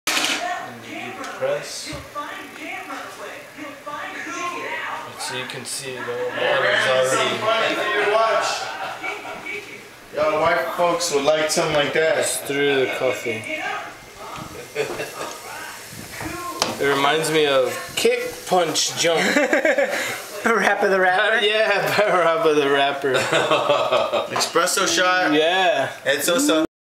Espresso Demo
Brannon demonstrates his new $29 coffee maker. The jokes about it being a breast pump or penis pump have already been made, so don't bother.